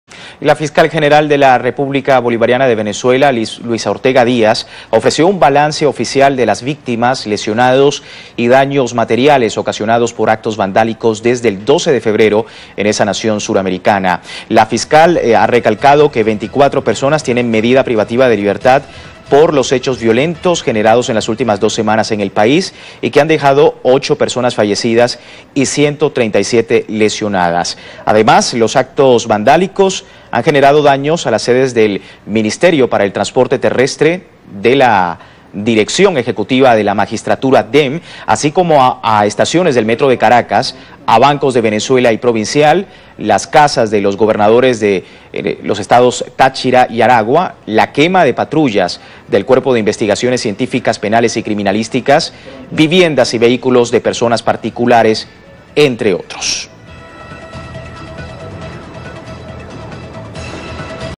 Números oficiais dos atos violentos na Venezuela
Acompanhe os números oficiais dos atos violentos da Venezuela desde o dia 12 de fevereiro, fornecidos pela Fiscal Geral da República, Luisa Ortega Díaz. teleSUR